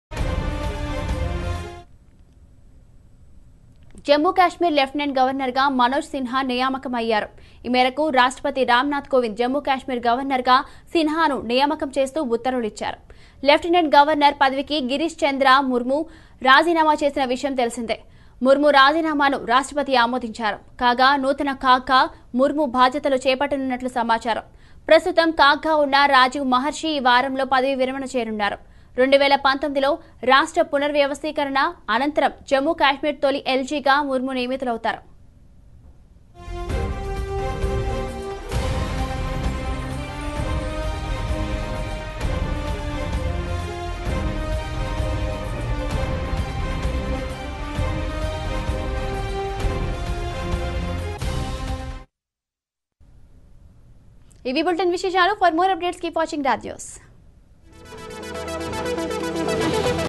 కొత్త గవర్నర్ | Manoj Sinha Appointed J&K Lieutenant Governor After GC Murmu Resigns | RAJ NEWS
కొత్త గవర్నర్ | Manoj Sinha Appointed J&K Lieutenant Governor After GC Murmu Resigns | RAJ NEWS TELUGU